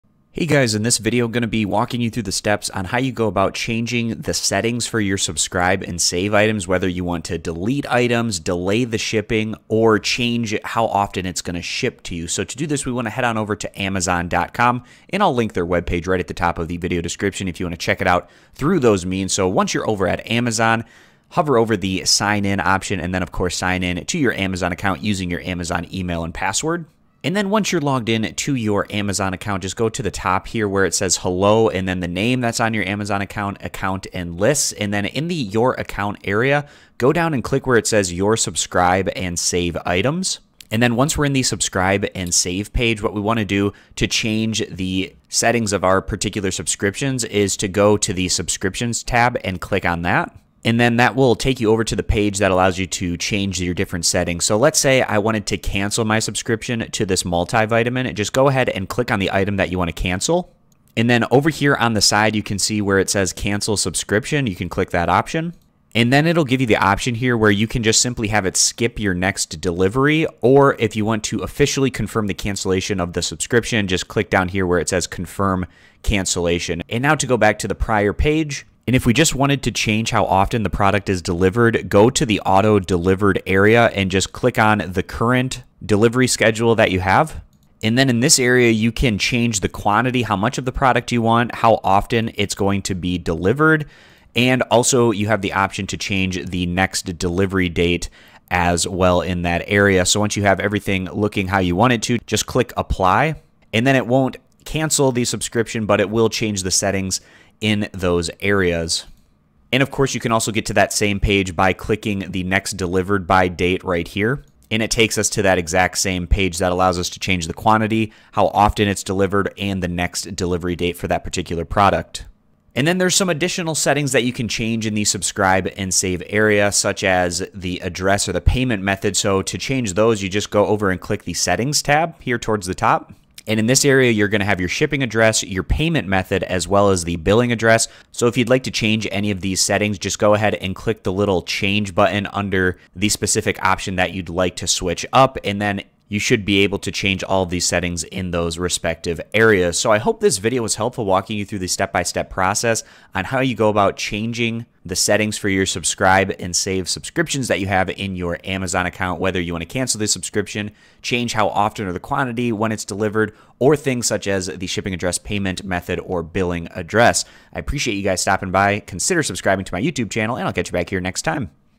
Amazon Subscribe and Save Cancel - Skip Delivery - How To Unsubscribe - How To Delete Instructions
I also touch on How To Change Amazon Subscribe and Save Settings, How To Unsubscribe Amazon Subscribe and Save, How To Delete Amazon Subscribe and Save, and Amazon Subscribe and Save How To Skip Delivery

In this video I walk through the step by step guide, instructions, tutorial on how to change the auto-delivery frequency, the next delivery date, your shipping address, billing address, payment method, or subscribe and save subscription items in your Amazon account. I hope this video was helpful.